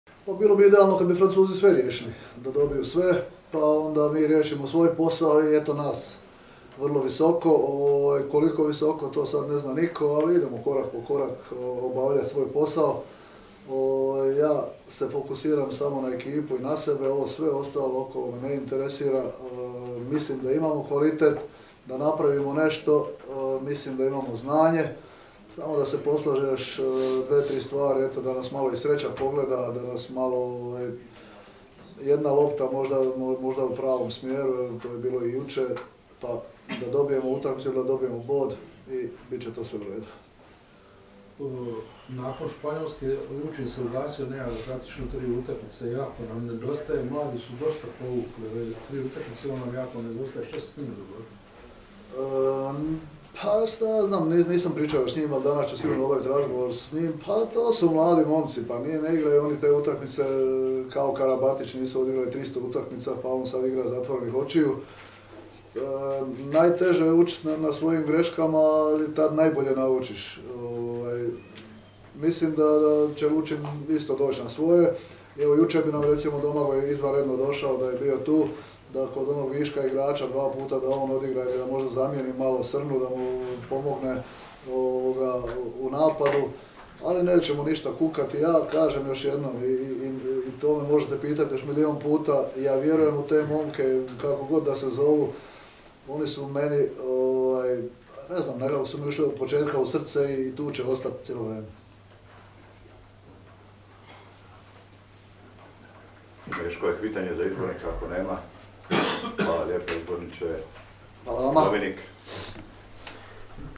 Perkovac: "Ja vjerujem u ove momke. Ušli su mi u srce i tu će uvijek ostati"
Konferencija za medije izbornika hrvatske rukometne reprezentacije Gorana Perkovca uoči utakmice Hrvatske protiv Mađarske na EP-u u Njemačkoj.